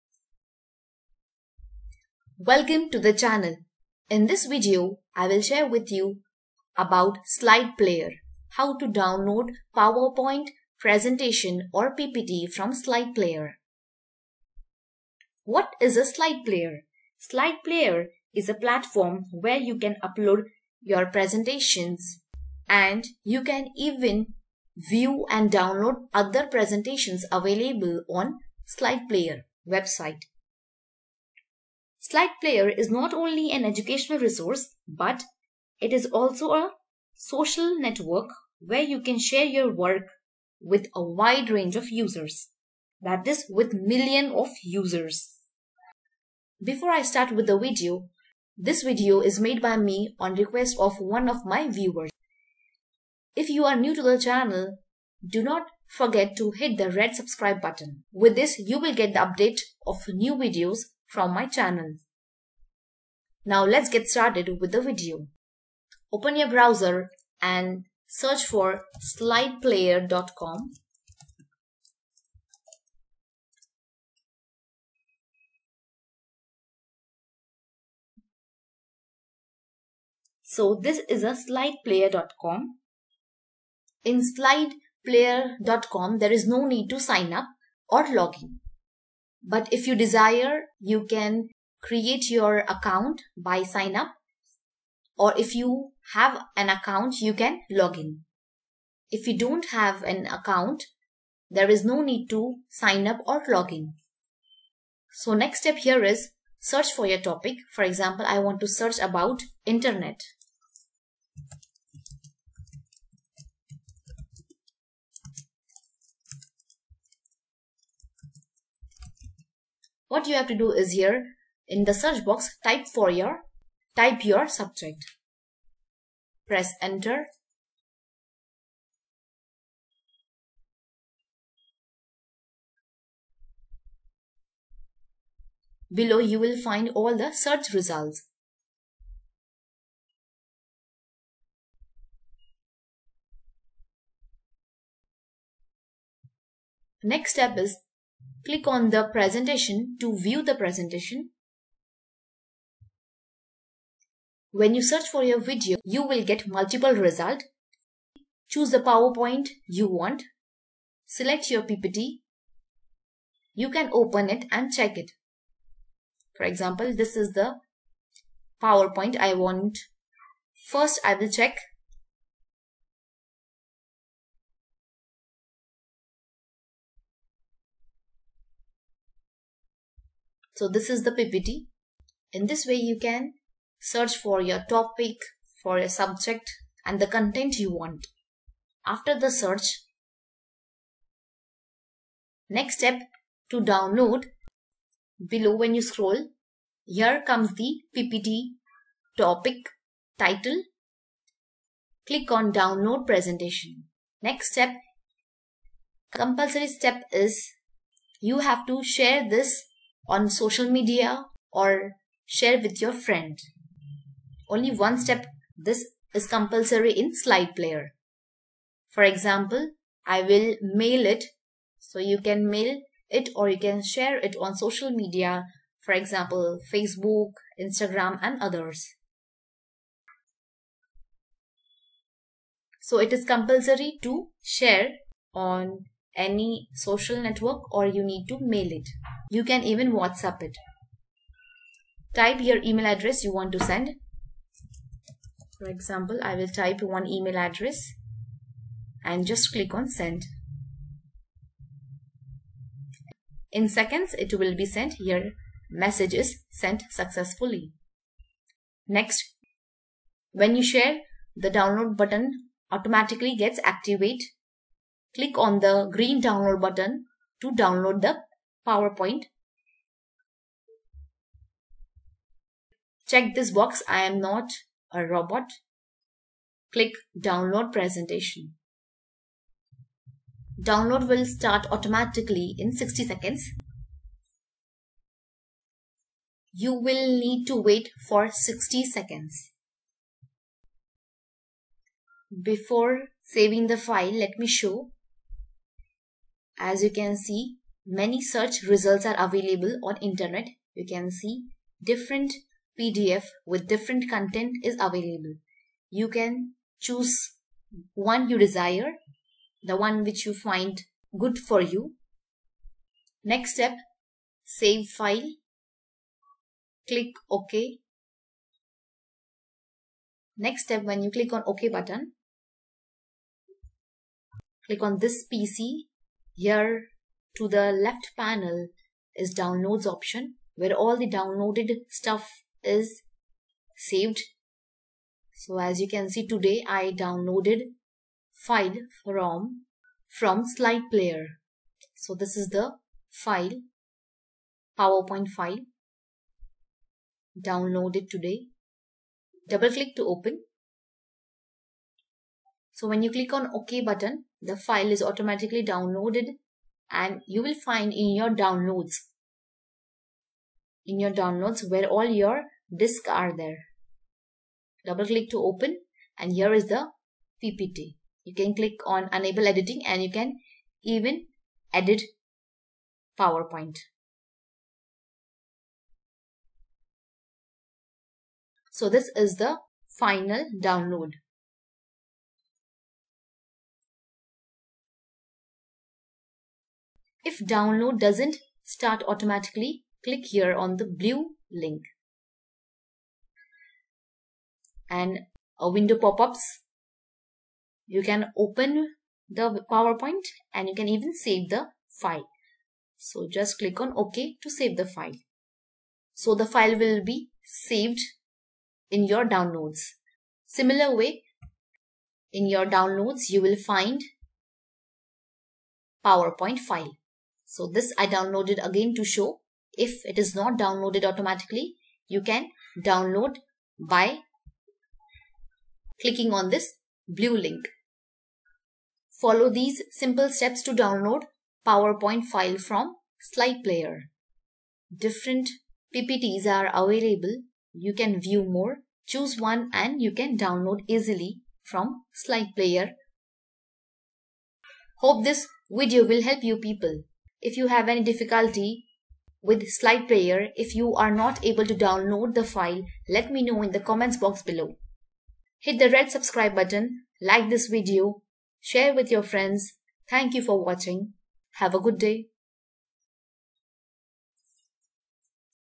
How to download ppt from SlidePlayer
This tutorial covers how to download PowerPoint from Slide Player.